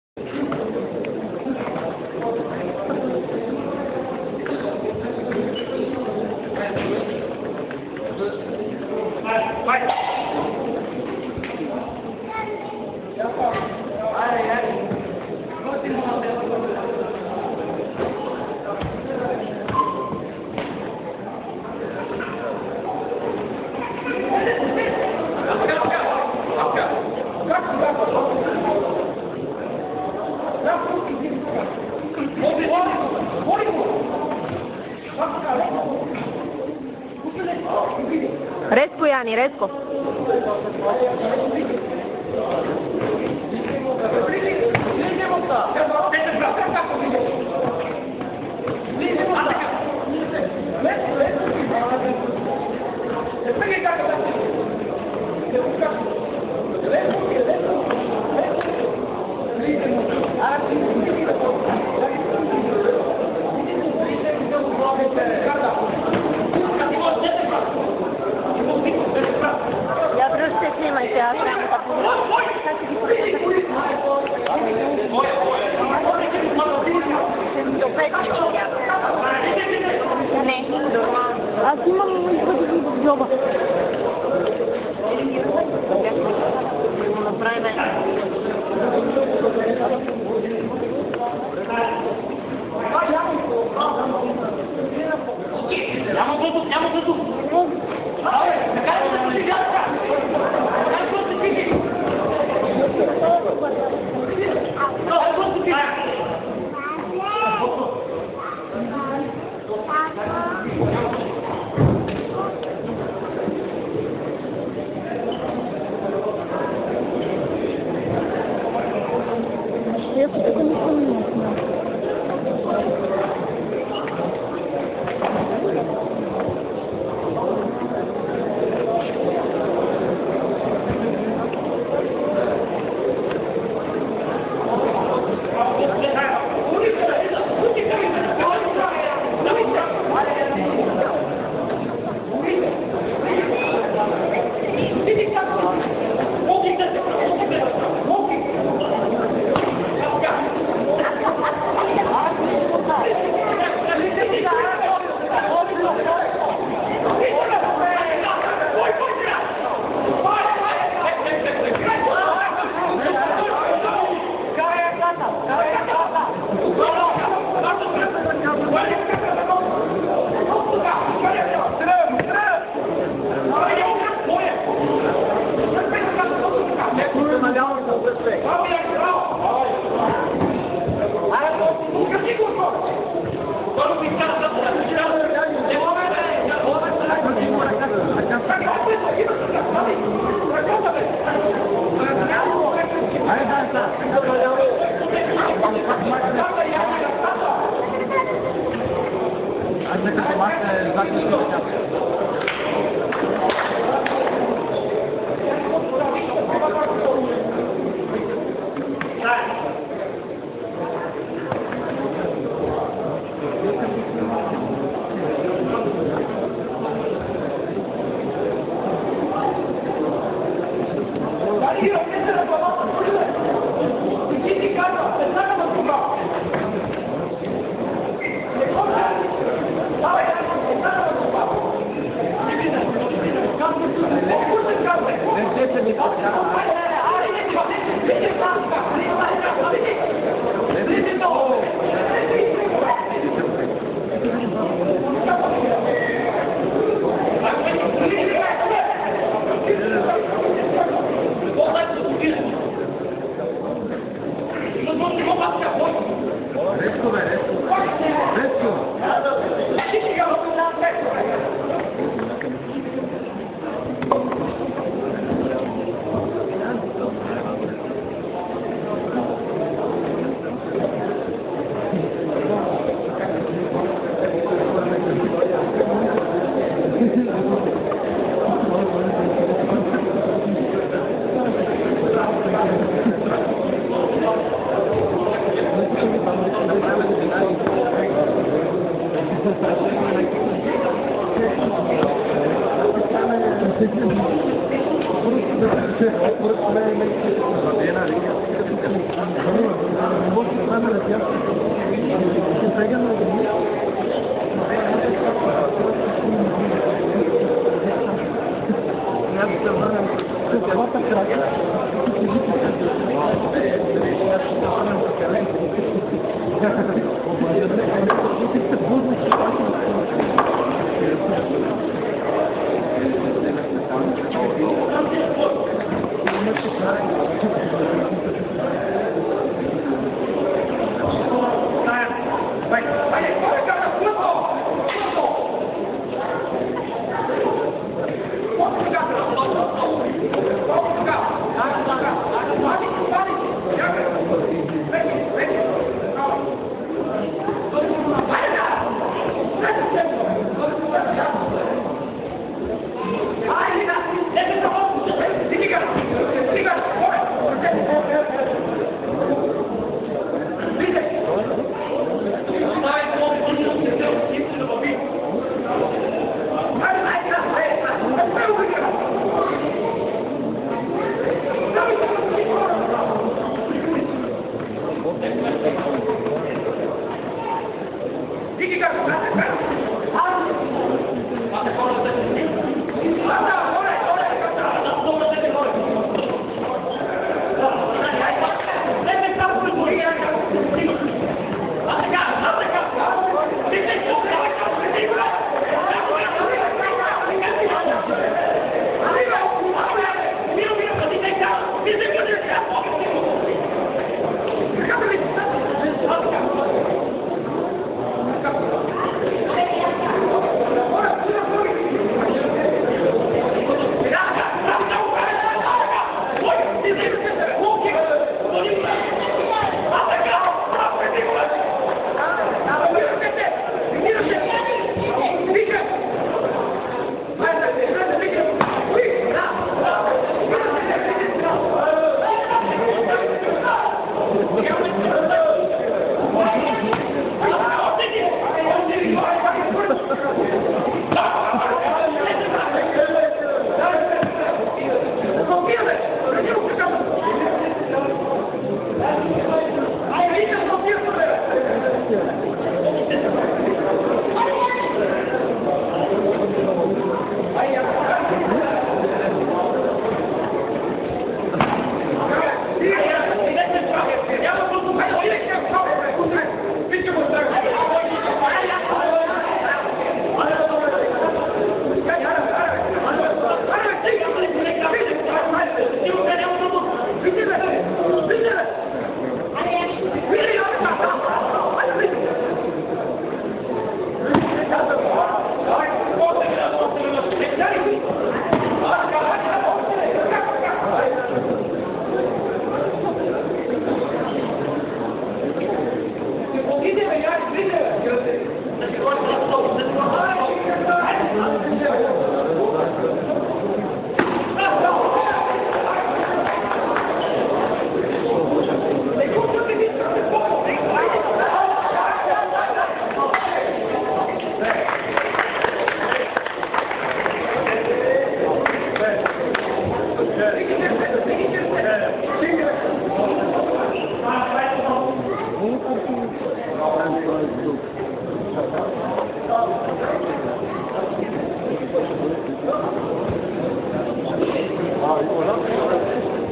02-12.06.2010 MMAA Yany Veckov VS Panteley Gergov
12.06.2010 MMA Amateur rules. Dupnica, Bulgaria